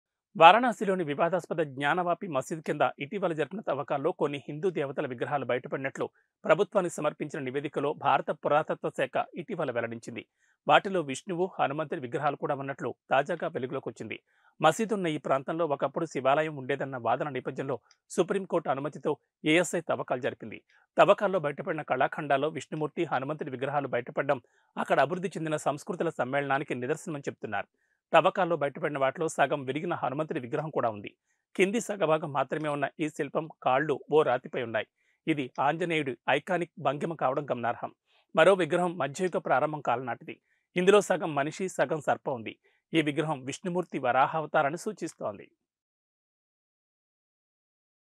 జ్ఞానవాపి మసీదు కాంప్లెక్స్‌లో దొరికిన విష్ణుమూర్తి, హనుమంతుడి విగ్రహాలు
జ్ఞానవాపి మసీదు కాంప్లెక్స్‌లో దొరికిన విష్ణుమూర్తి, హనుమంతుడి విగ్రహాలు | @KarimnagarPuraLocal

వారణాసిలోని వివాదాస్పద జ్ఞానవాపి మసీదు కింద ఇటీవల జరిపిన తవ్వకాల్లో కొన్ని హిందూ దేవతల విగ్రహాలు బయటపడినట్టు ప్రభుత్వానికి సమర్పించిన నివేదికలో భారత పురతత్వశాఖ (ఏఎస్ఐ) ఇటీవల వెల్లడించింది. వాటిలో విష్ణువు, హనుమంతుడి విగ్రహాలు కూడా ఉన్నట్టు కూడా తాజాగా వెలుగులోకి వచ్చింది. మసీదు ఉన్న ఈ ప్రాంతంలో ఒకప్పుడు శివాలయం ఉండేదన్న వాదన నేపథ్యంలో సుప్రీంకోర్టు అనుమతితో ఏఎస్ఐ తవ్వకాలు జరిపింది.